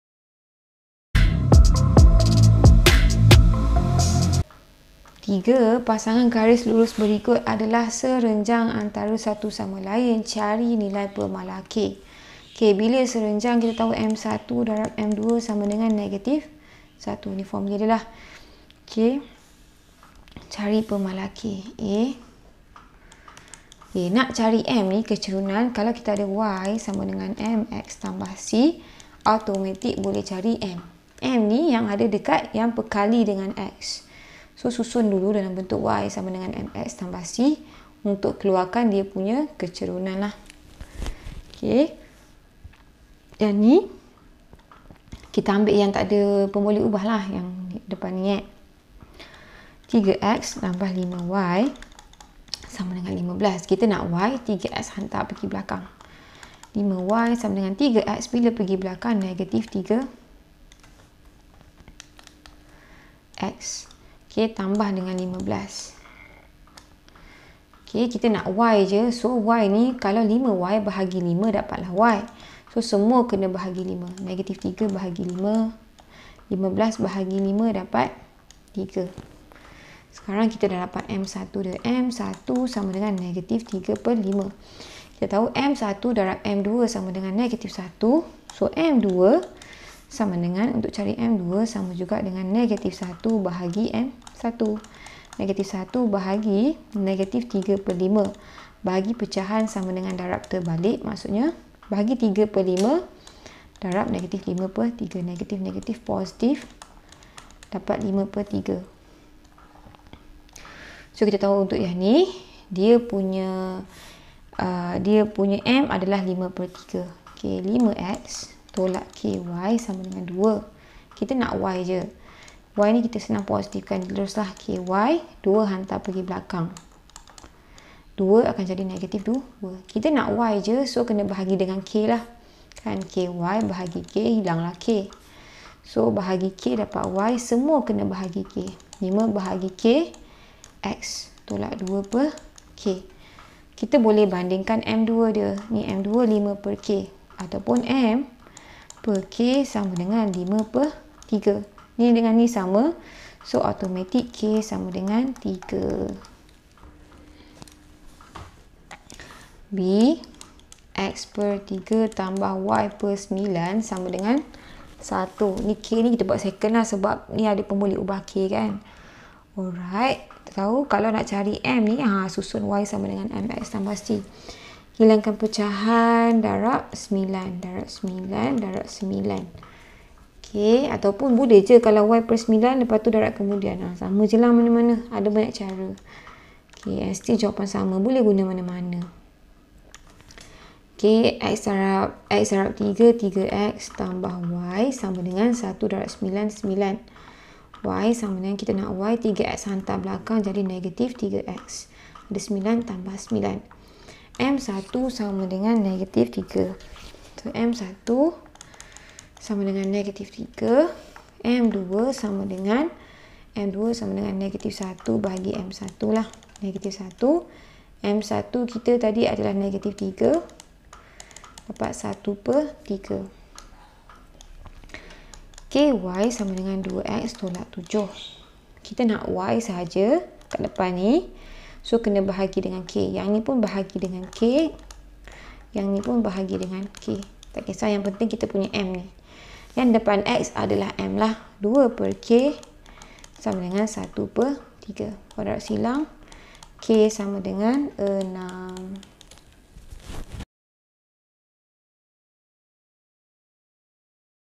Latih diri 7.4 Soalan 3 | Garis lurus selari dan garis lurus serenjang | Bab 7 Geometri Koordinat
Latih diri 7.4 Soalan 3 | Garis lurus selari dan garis lurus serenjang | Bab 7 Geometri Koordinat | Add Maths F4 | Matematik Tambahan Tingkatan 4 | Add Maths Form 4

Pasangan garis lurus berikut adalah berserenjang antara satu sama lain. Cari nilai pemalar k.